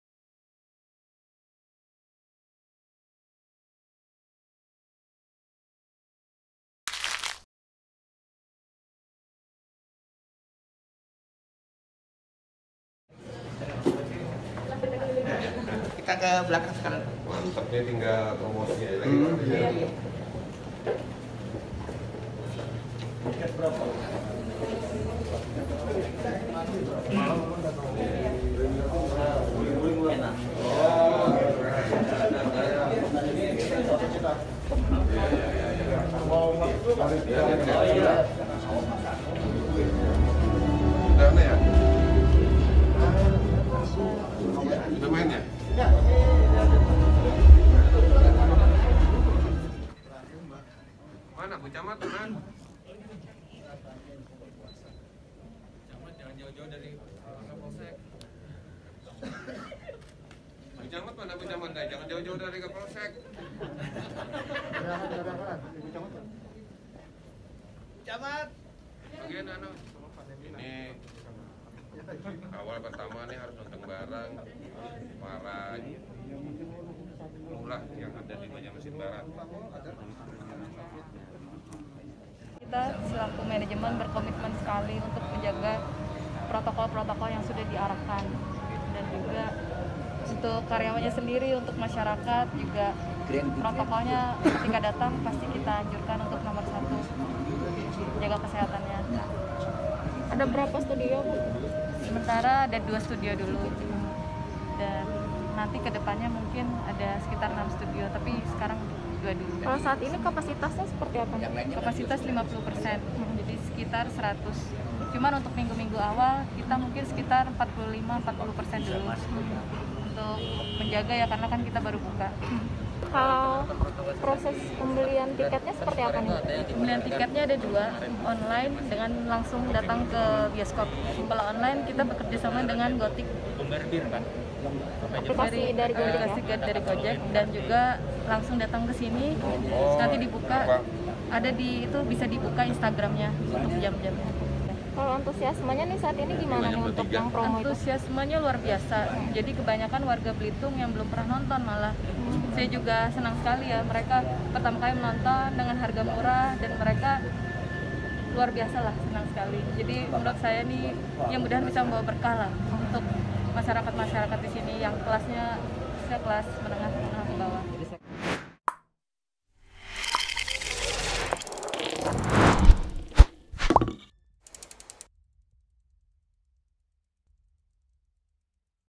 Alternatif Bioskop Baru Di Banjarmasin, KCM Belda Incar Pengunjung Menengah Ke Bawah
BANJARMASINPOST.CO.ID, BANJARMASIN - Geliat fasilitas hiburan kian marak di era new normal yang mana masih berdampingan dengan pandemi Covid-19. Masyarakat Banjarmasin dan sekitarnya disuguhkan alternatif bioskop baru yakni Kota Cinema Mall (KCM) Belda Banjarmasin.

Tak seperti bioskop sebelumnya yang menyatu dengan mall, bioskop ini berkonsep mandiri atau berupa gedung sendiri yang dilengkapi fasilitas minuman dan juga makanan ringan. KCM Belda berlokasi di Jalan Belitung Darat No 187 Banjarmasin.

Pimpinan KCM Belda dan Jumpa Square Banjarmasin, Christin Chandra mengungkapkan, animo warga Banjarmasin khususnya di sekitaran KCM Belda cukup tinggi untuk nonton bioskop.

"Antusias masyarakat di Belitung Darat sangat luar biasa terlebih beberapa ada yang belum pernah nonton bioskop, adanya promo nonton film Rp 10.000 di masa opening sangat membantu mereka menikmati film bioskop dengan harga terjangkau. Semoga menjadi berkah khususnya bagi masyarakat menengah ke bawah," ucapnya usai Grand Opening KCM Belda Banjarmasin kepada Banjarmasinpost.co.id, Senin (2/11/2020).

Diskon spesial Rp10.000 tersebut berlaku untuk film Indonesia yang berjudul "Aku Tahu Kapan Kamu Mati" dibintangi Natasya Wilona yang dapat disaksikan mulai 31 Oktober hingga 6 November 2020.

Kemudian dalam pelaksanaannya, kcm Melda Banjarmasin telah mengantongi izin dan rekomendasi dari Pemerintah Kota (Pemko) Banjarmasin dan Gugus Tugas Covid-19.

Sementara, Bussines Unit Manager KCM Belda, Farouk Pratama menjelaskan, selama masa opening KCM Belda mengoperasikan dua studio bioskop, menayangkan tiga film pada akhir pekan dan dua film di hari kerja.

"Kapasitas normal sebanyak 205 seat, namun selama masa pandanganmu hanya membuka kapasitas sebesar 40-45 perse. dan nantinya perlahan hingga 50 persen," paparnya.

Proses pembelian ada dua jenis transaksi yakni transaksi online melalui aplikasi Go-Tix, dan offline yang mana pengunjung bisa langsung beli tiket di KCM Belda.

Acara grand opening dibuka oleh Plt Wali Kota Banjarmasin, Hermansyah disaksikan manajemen KCM Belda, manajemen KCM pusat dan jajaran Pemko Banjarmasin. (Banjarmasinpost.co.id/Mariana)